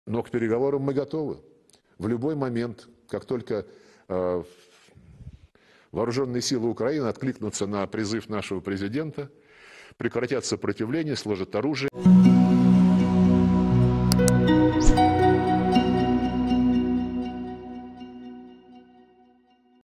Russia ready to talk with Ukraine if army 'lays down arms': Lavrov
Russian Foreign Minister Sergei Lavrov says Moscow is ready for talks with Kyiv if the Ukrainian army surrenders, as Russian invading forces advance on the capital. "We are ready for negotiations at any moment, as soon as the armed forces of Ukraine respond to our call and lay down their arms," Lavrov says at a press conference in Moscow, after talks with his counterparts from the self-proclaimed Donetsk People's Republic and Lugansk People's Republic